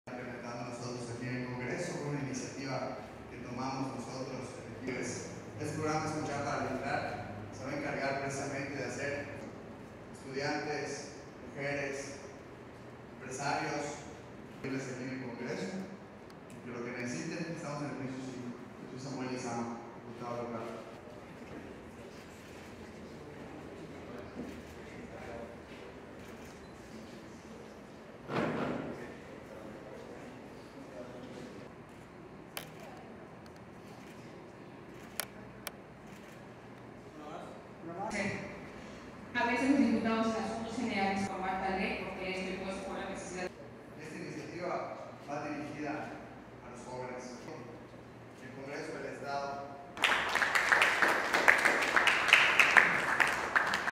El Congreso brinda la oportunidad a los jóvenes estudiantes para conocer el trabajo legislativo
Poder Legislativo de puertas abiertas a la niñez y las juventudes.

Mérida, Yucatán a 16 de octubre de 2024.- Un grupo de estudiantes de la Licenciatura en Derecho de la Universidad Modelo visitó las instalaciones del recinto del Congreso del Estado con la finalidad de fomentar el interés en cuanto al trabajo que realizan las y los diputados.

El Instituto de Investigaciones Legislativas, que está a cargo del Maestro Erik Ojeda Novelo, informó que este Congreso será de puertas abiertas a los estudiantes de todos los niveles educativos del estado, que estén interesados en conocer las acciones que se realizan de manera habitual en el Poder Legislativo, como las sesiones plenarias, comisiones, y otras actividades.

En esta ocasión, los jóvenes visitantes fueron recibidos por el Diputado Samuel Lizama Gasca (MORENA), quien aprovechó informar a los estudiantes de Derecho de la Universidad Modelo, el programa “Escuchar para Legislar” cuya finalidad será impulsar la participación y las iniciativas ciudadanas de manera sustantiva.

Personal del Congreso realizó una presentación para explicar a las y los jóvenes la línea a seguir para la elaboración de iniciativas, así como los procesos realizados para la generación del producto legislativo y su aprobación en el Pleno.

Posteriormente, visitaron la Biblioteca “Fidelia Sánchez Mendiburu” y la Sala de Sesiones “Constituyentes de 1918”.

El Presidente de la Junta de Gobierno y Coordinación Política del Congreso del Estado, Diputado Wilmer Monforte Marfil acompañado de la Presidenta de la Mesa Directiva, Diputada Neyda Pat Dzul y del Director del Instituto de Investigaciones Legislativas, Erik Ojeda Novelo agradecieron su visita a este espacio donde presentan, discuten y aprueban las iniciativas de leyes que impactan directamente a la comunidad yucateca.